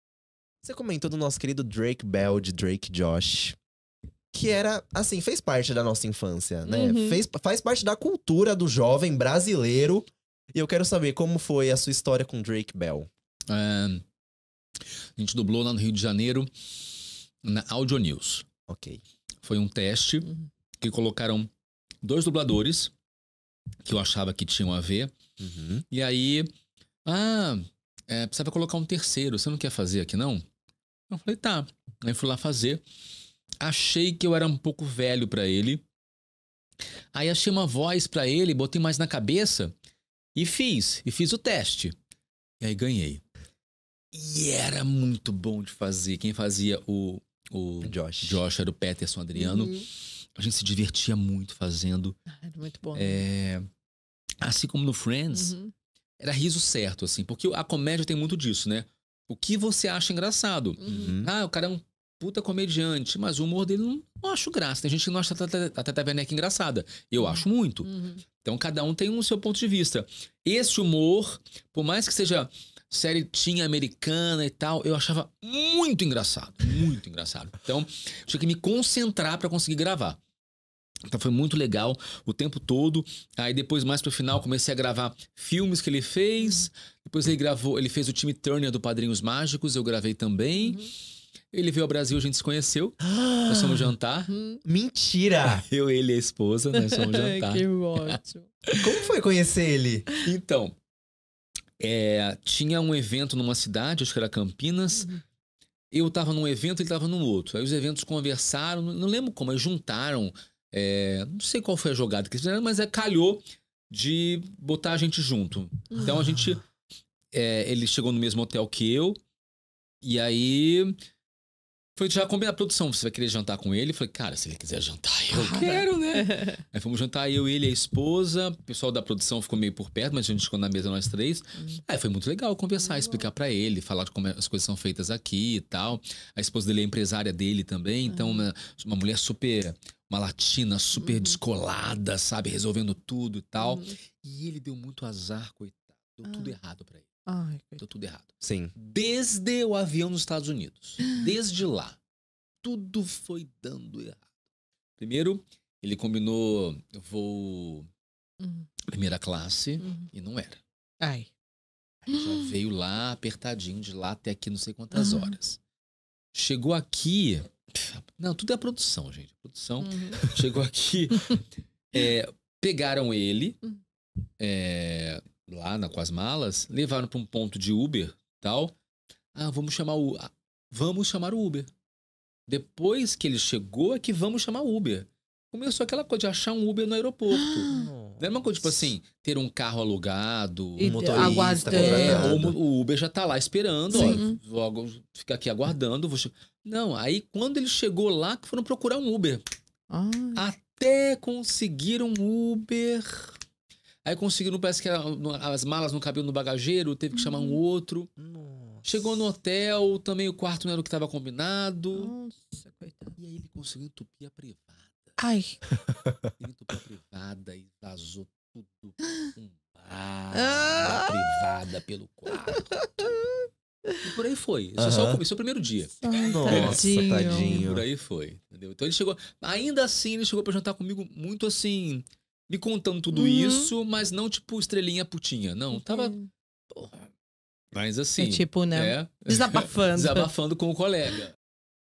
Felipe Grinnan conta do dia que conheceu o ator Drake Bell de Drake e Josh - CORTES POD-ATUAR #93
Seu podcast favorito sobre Atuação 🎭

Felipe Grinnan - Instagram: @FelipeGrinnan

🎙️Hosts (anfitriões):
Breno Oroz - Instagram: @Breno.Oroz

Mali -             Instagram: @Rosangela_malimpensa

SmileMe -  Instagram: @Smileme.aligner